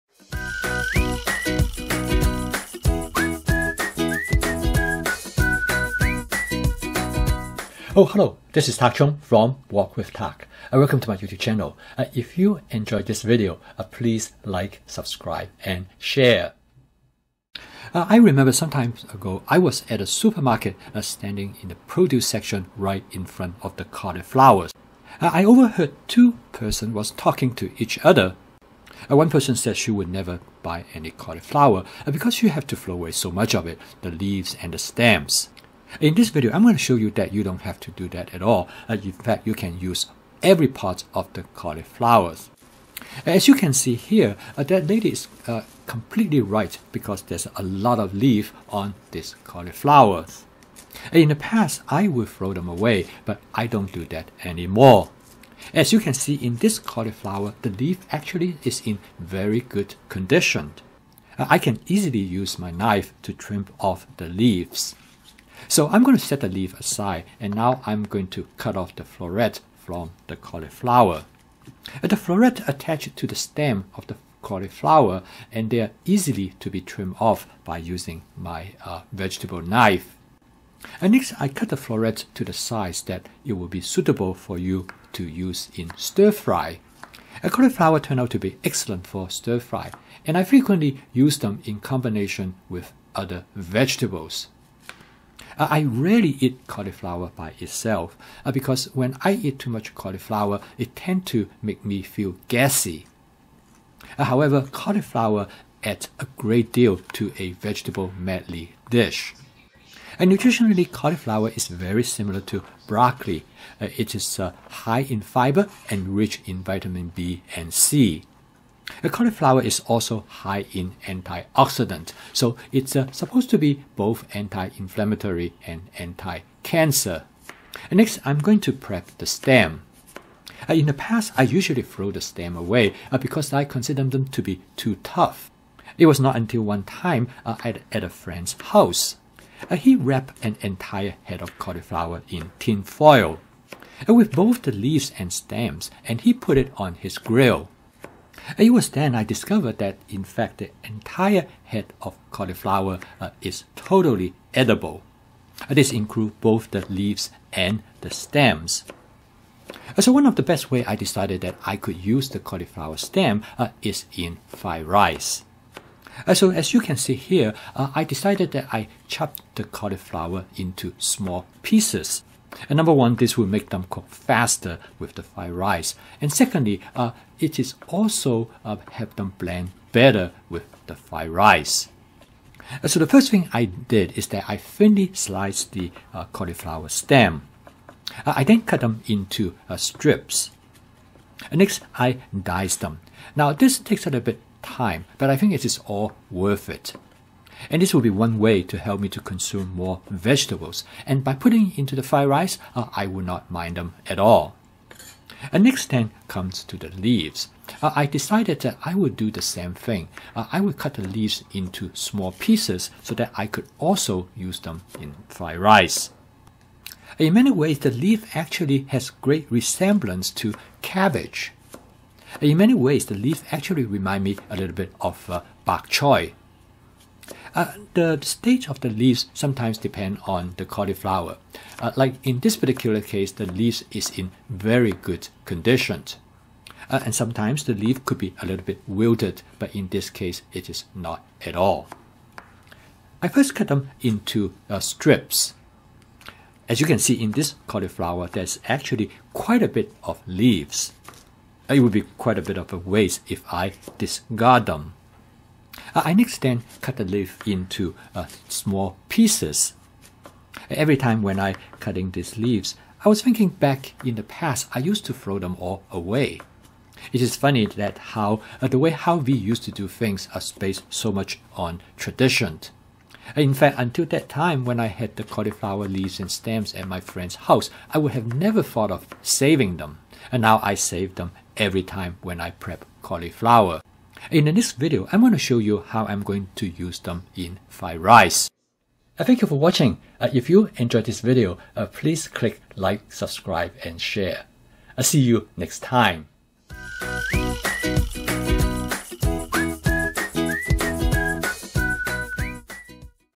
Prepping with Tak. How to make full use of a cauliflower
Over the years, I find that I get the biggest boost in my cooking is through inspiration of doing something that I had never done before and also did not realize that I could do it easily.  In this video, I would like to tell you one of these experiences.  For my entire life when I prepped cauliflower, I always discarded the leaves and stems.   I never gave it too much thought, because this was how it was done by other people whom I know.  It was not until about 5 years ago, I was eating at a friend's house.  He wrapped an entire head of cauliflower, leaves and stem, in aluminum foil and cooked it on his grill.  At dinner, I was surprised that every part of the cauliflower was edible and delicious.  This was an inspiration to me, because I like the idea that there would be no waste, and I would be able to find creative use for the stems and leaves.  The first thing that came to mind is the stems and leaves in a fried rice dish.  Chopping up the stems and the leaves was simple and straightforward.  However, good knife skills will help with the process.  It takes an extra 10 minutes to prep the stems and leaves, and I was happy to comply.  The cauliflower stems and leaves add great flavor and texture to the fried rice dish, which I will demonstrate in another video.